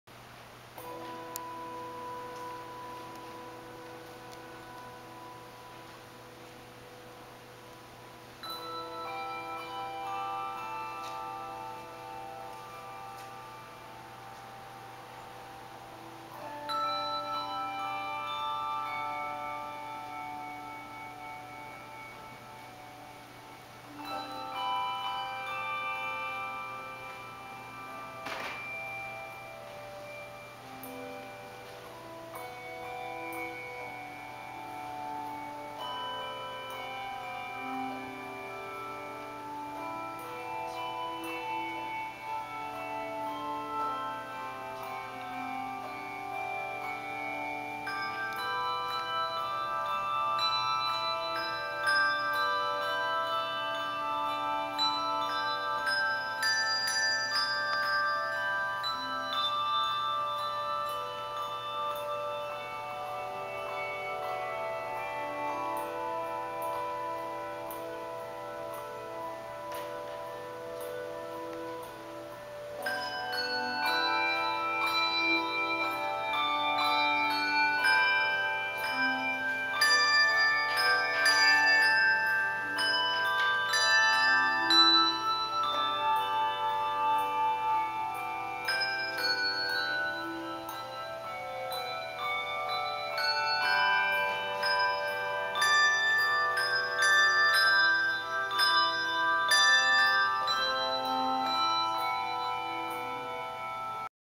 anthem handbells 2015 Feb 15
This opening minute sets the stage for the build up to the climax in the last 1/3 of the piece.